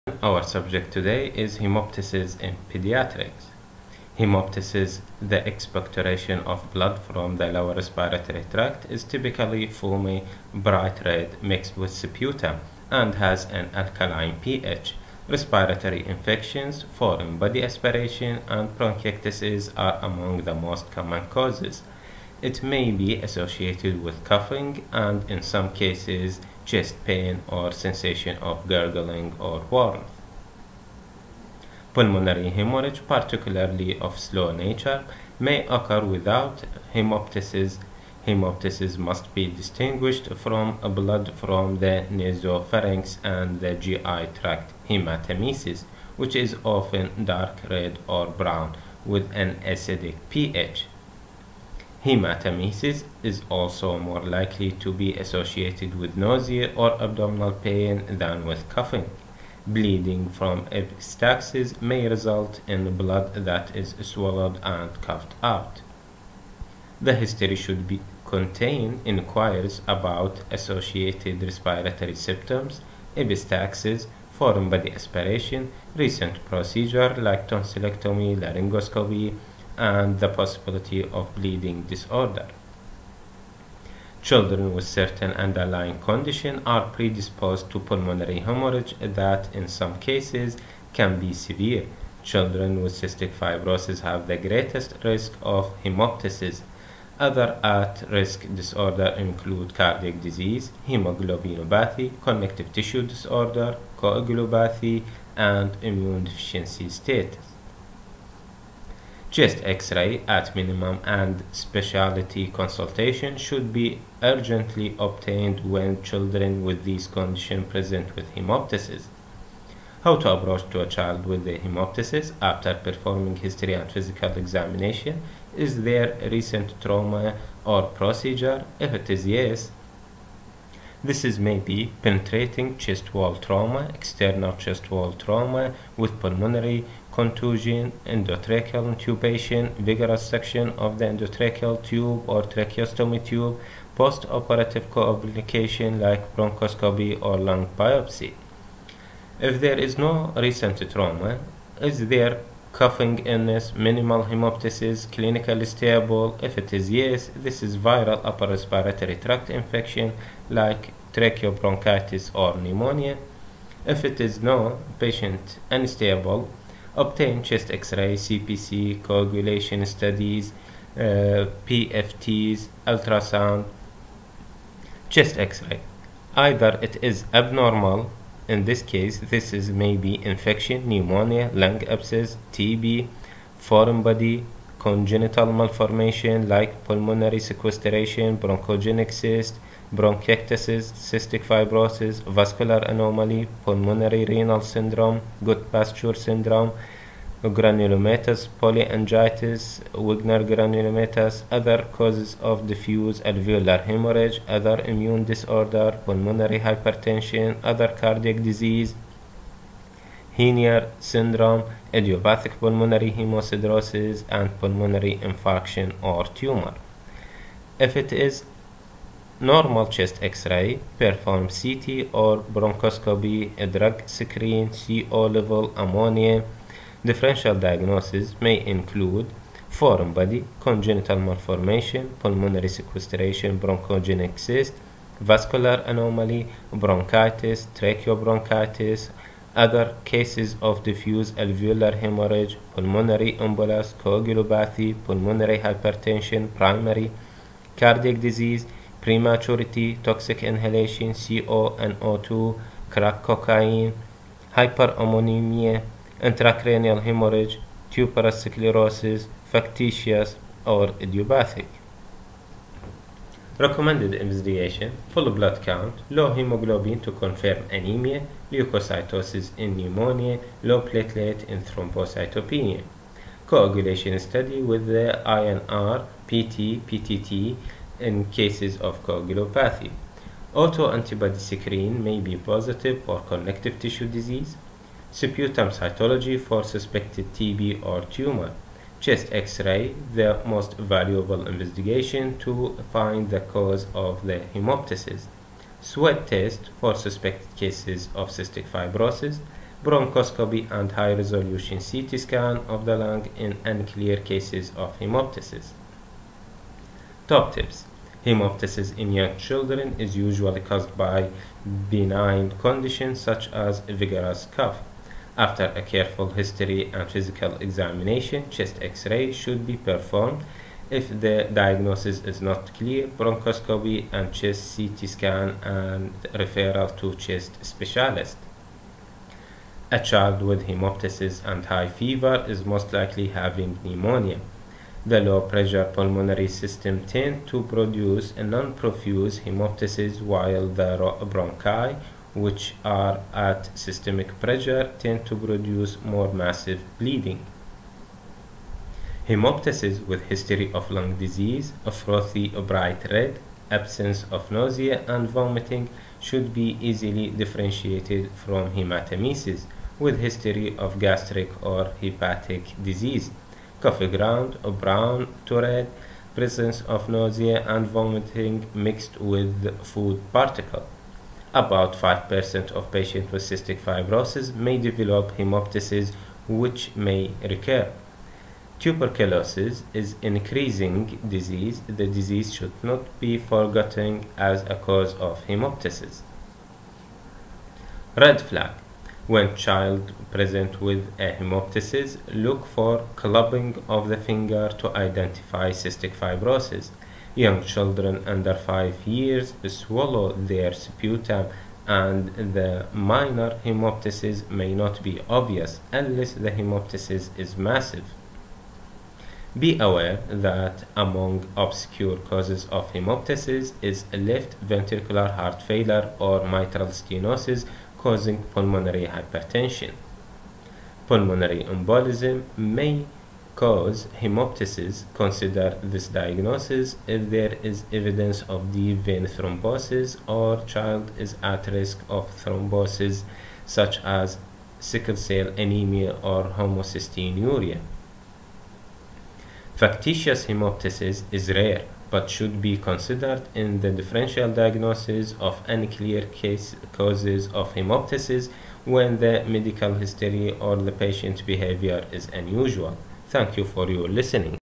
HEMOPTYSIS in pediatrics
how to approach to pediatric HEMOPTYSIS
references
pediatric decision-making strategies 2nd edition
paediatric symptom and sign sorter 2nd edition